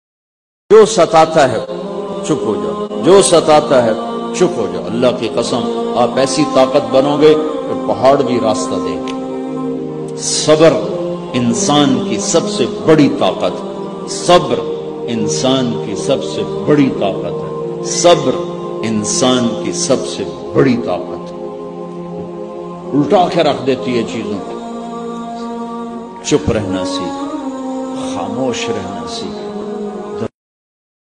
WhatsApp Status Sabar Insan Ki Sub Sai Bari Taqat Hai By Molana Tariq Jameel
Sabar Insan Ki Sub Sai Bari Taqat Hai

WhatsApp Status